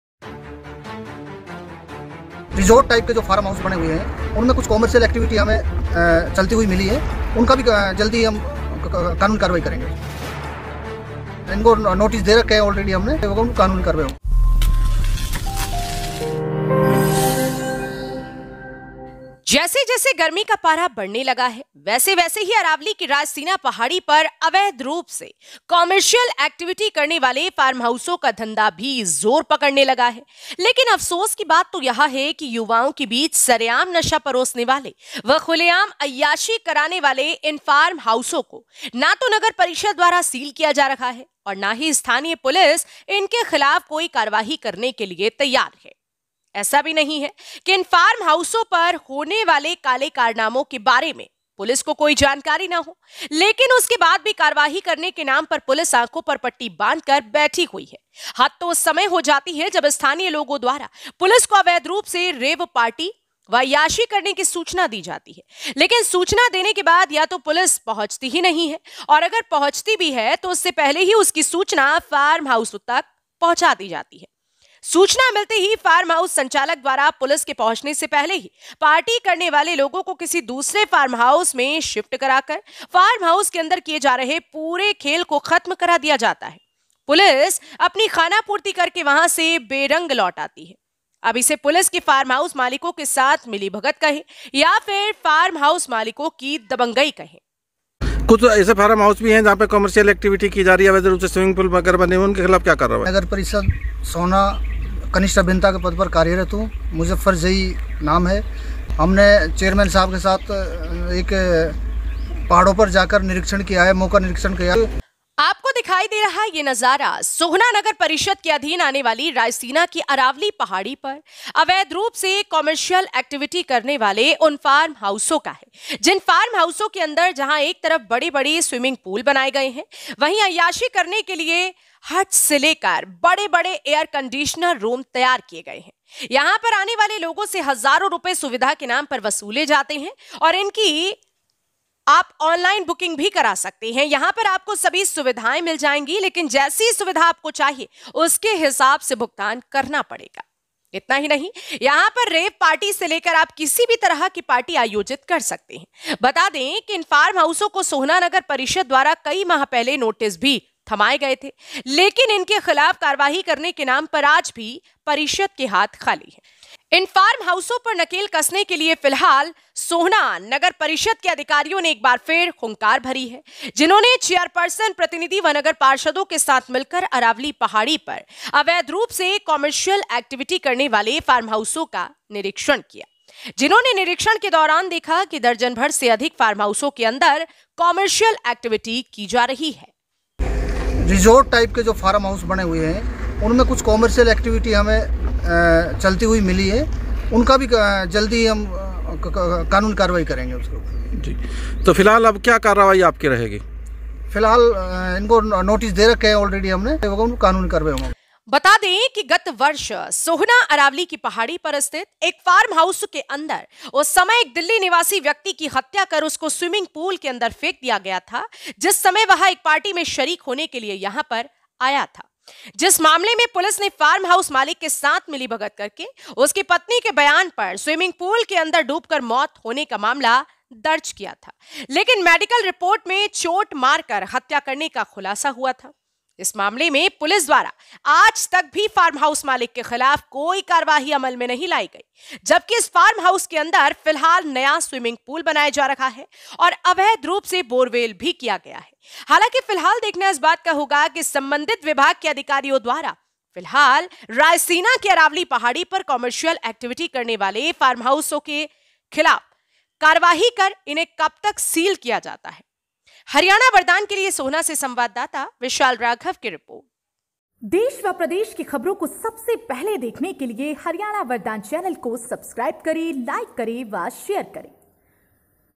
Sohna|"आखिर कौन करेगा अय्यासी के अड्डों पर कार्यवाही,अरावली की चोटी पर पहुँच रहे शबाब के शौकीन..
जैसे-जैसे गर्मी का पारा बढ़ने लगा है वैसे-वैसे ही अरावली की रायसीना पहाड़ी पर अवैध रूप से कॉमर्शियल एक्टिविटी करने वाले फार्म हाउसों का धंधा भी जोर पकड़ने लगा है..लेकिन अफसोस कि बात तो यह है..युवाओं के बीच सरेआम नशा परोसने वाले व खुले आम अय्यासी कराने वाले इन फार्म हाउसों को ना तो नगर परिषद द्वारा सील किया जा रहा है,और ना ही स्थानीय पुलिस इनके खिलाफ कोई कार्यवाही करने के लिए तैयार है..ऐसा भी नही है कि इन फार्म हाउसों पर होने वाले काले कारनामो के बारे में पुलिस को कोई जानकारी ना हो..लेकिन उसके बाद भी कार्यवाही करने के नाम पर पुलिस आंखों पर पट्टी बांधकर बैठी हुई है..हद तो उस समय हो जाती है जब स्थानीय लोगो द्वारा पुलिस को अवैध रूप से रेव पार्टी व अय्यासी करने की सूचना दी जाती है..लेकिन सूचना देने के बाद या तो पुलिस पहुँचती ही नही है,और अगर पहुचती भी है तो उससे पहले ही उसकी सूचना फार्म हाउस तक पहुँचा दी जाती है..सूचना मिलते ही फार्म हाउस संचालक द्वारा पुलिस के पहुचने से पहले ही..पार्टी करने वाले लोगो को किसी दूसरे फार्म हॉउस के में शिफ्ट करा कर फार्म हाउस के अंदर किए जा रहे पूरे खेल को खत्म करा दिया जाता है.. पुलिस अपनी खाना पूर्ति करके वहां से बेरंग लौट आती है अब इसे पुलिस की फार्म हाउस मालिको के साथ मिली भगत कहे या फिर फार्म हाउस मालिको की दबंगई कहे..